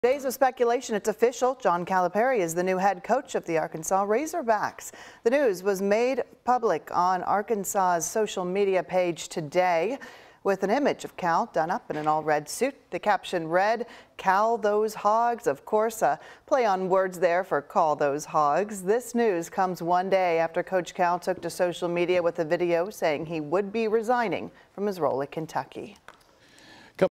It's official, Coach Cal named head coach at Arkansas
This comes after days of speculation.